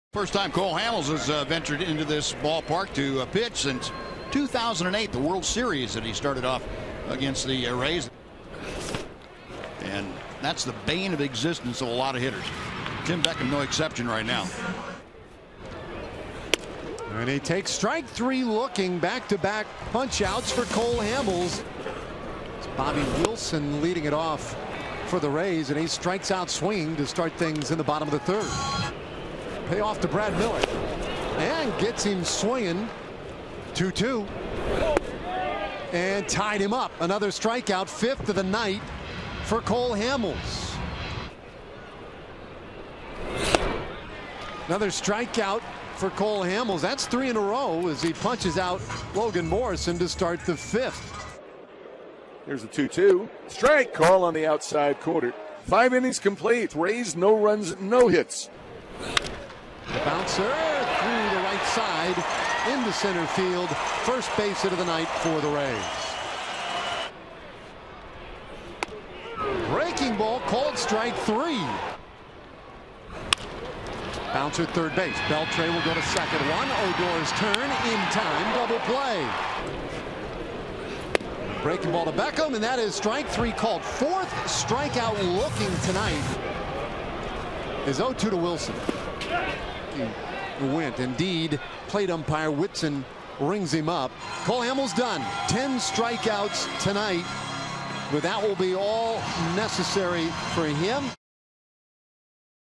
TEX@TB: Hamels fans 10 over 7 1/3 frames in the win
Cole Hamels fires 7 1/3 dominant innings, holding the Rays to one run on three hits with 10 strikeouts in the Rangers' 6-2 win over the Rays